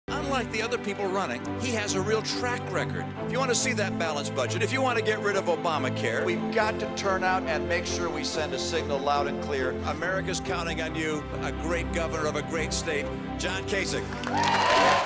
Please Caucus for Gov. John Kasich
On Tuesday, March 22nd - please caucus for Gov. John Kasich in Utah.